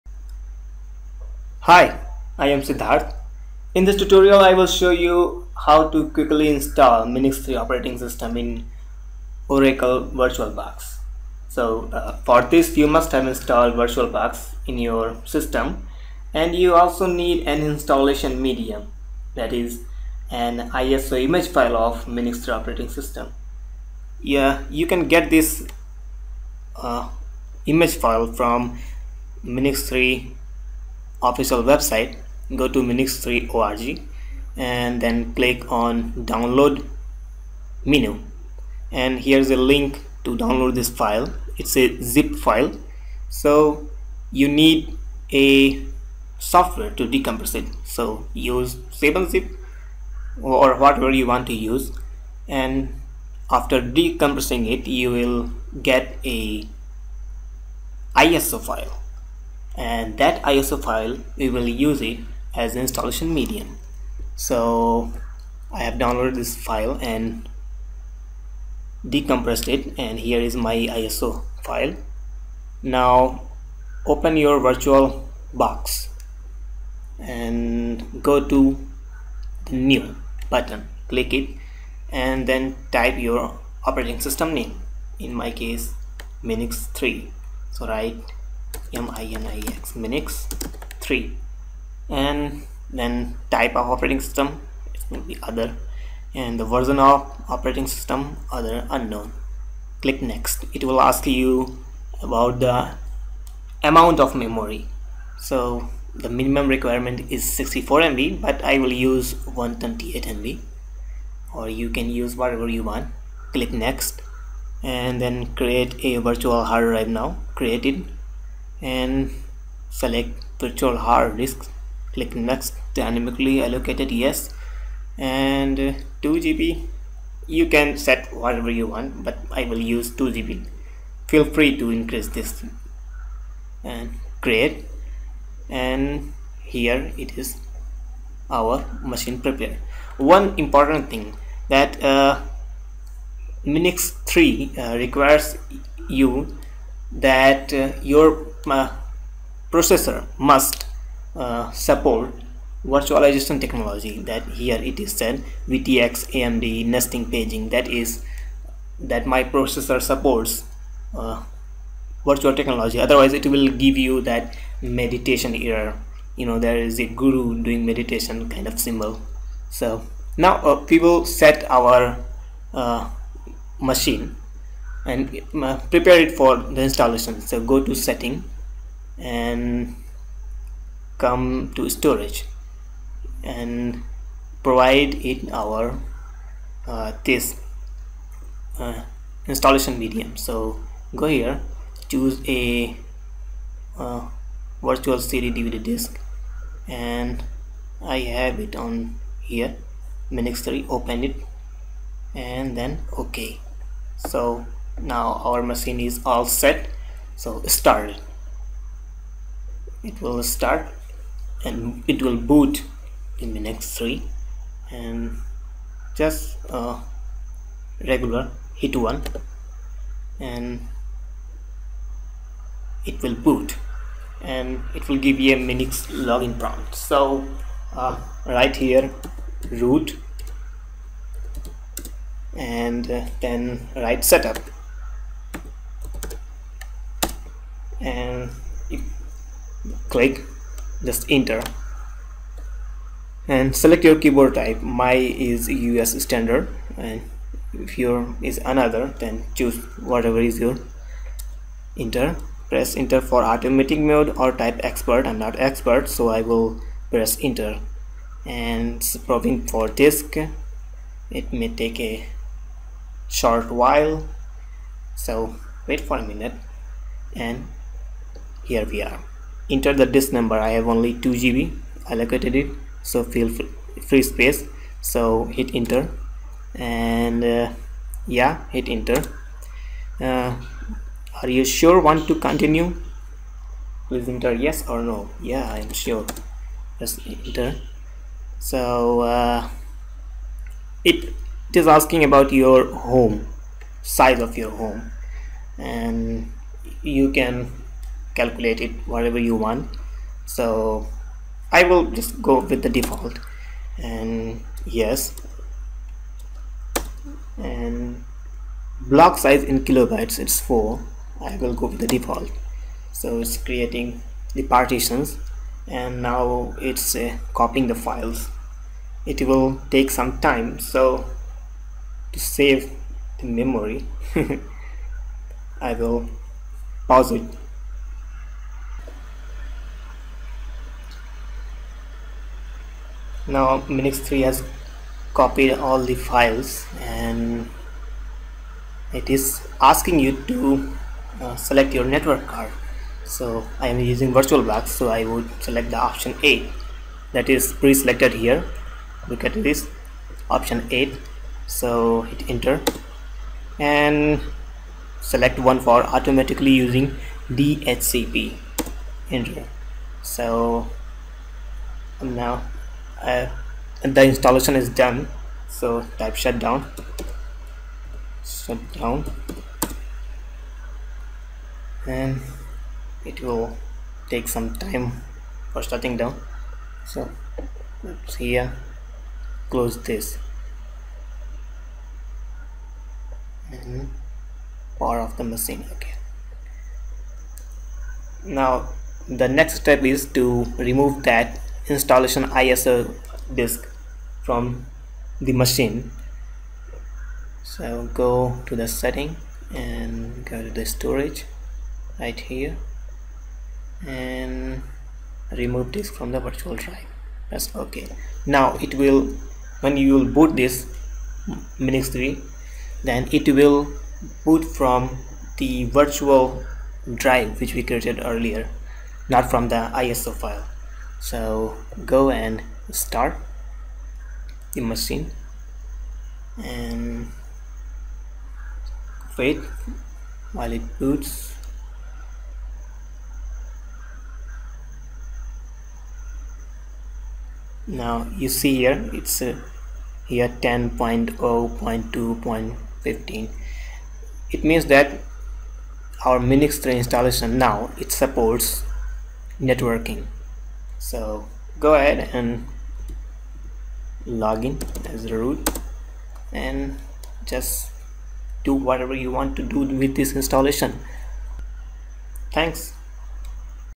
Install MINIX 3 in Virtualbox with Networking
Install MINIX 3 operating system in your Oracle Virtualbox with networking support.